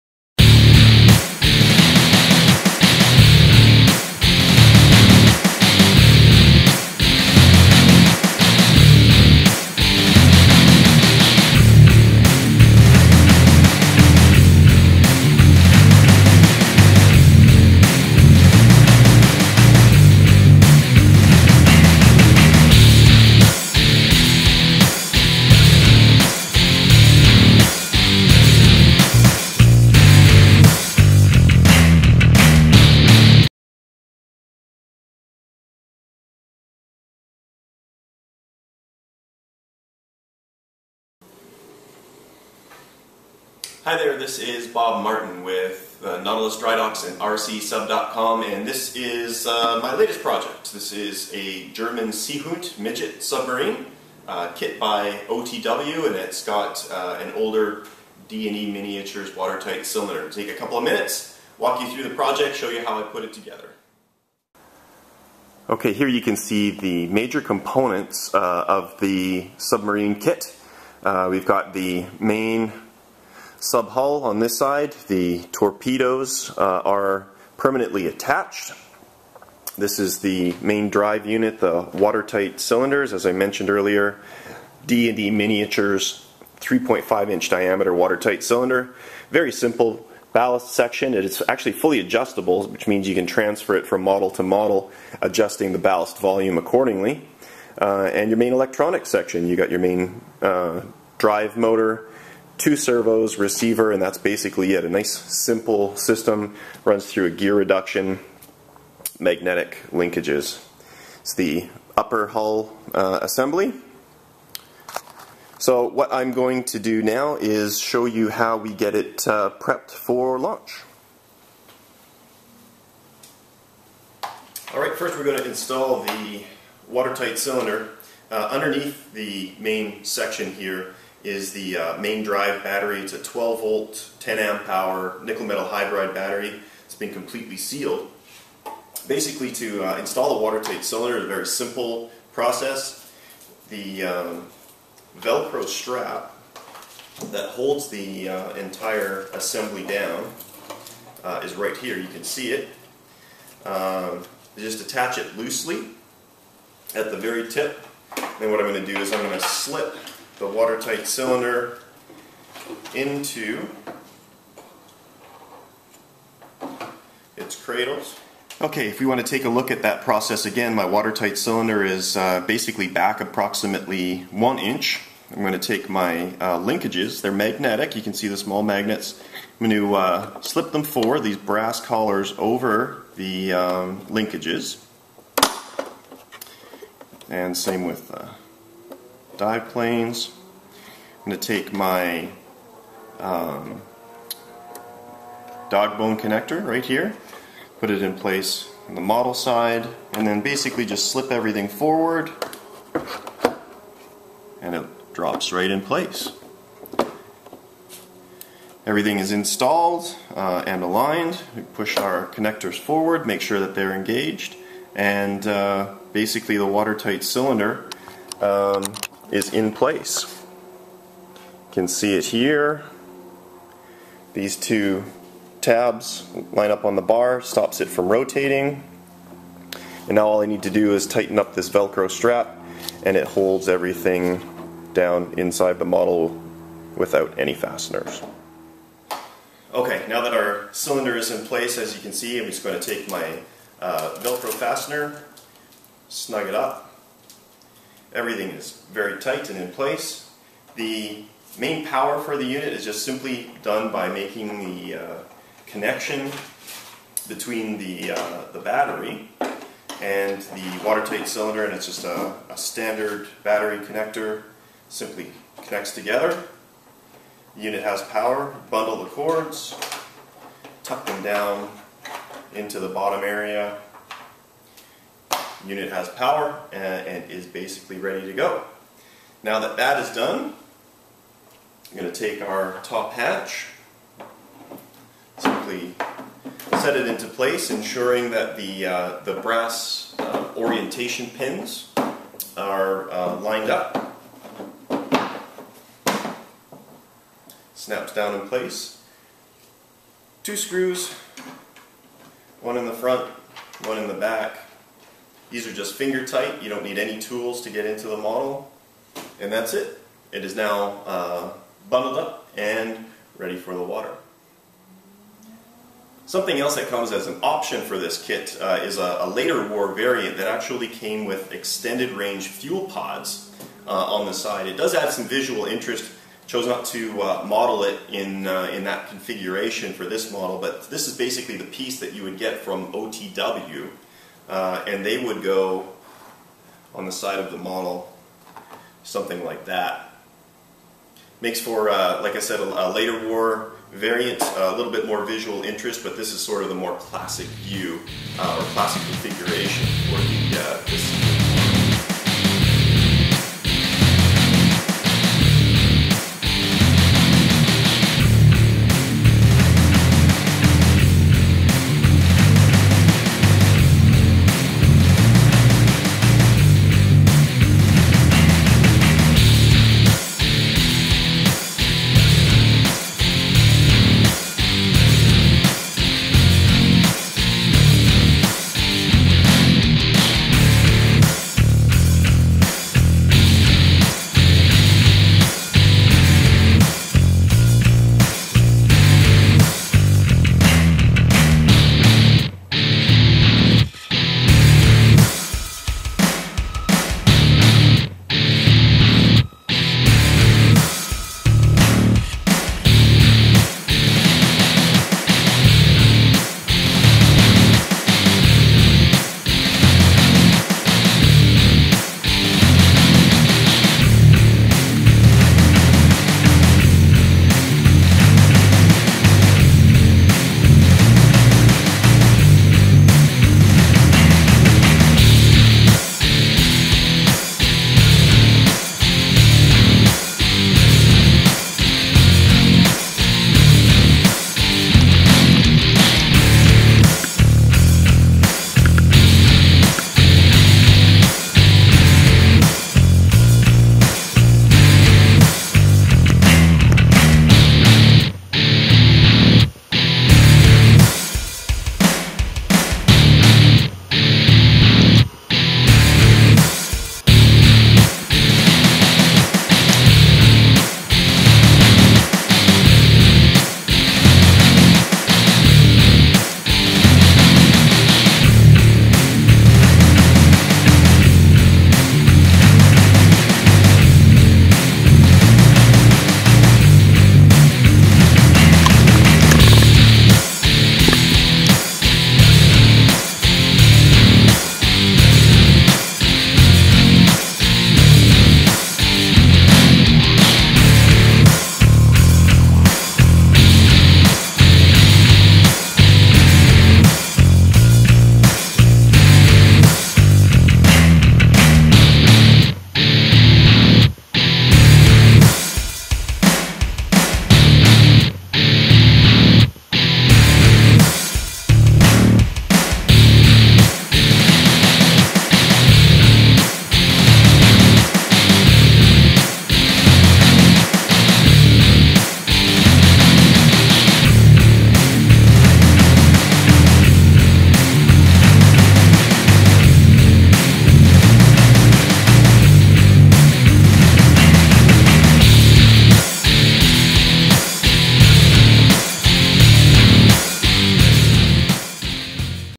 German Seehund R/C Submarine
This is my build of a German Seehund mini submarine.  The hull is from OTW in the the UK and it has an older D&E Miniatures WTC that the customer supplied.  Enjoy!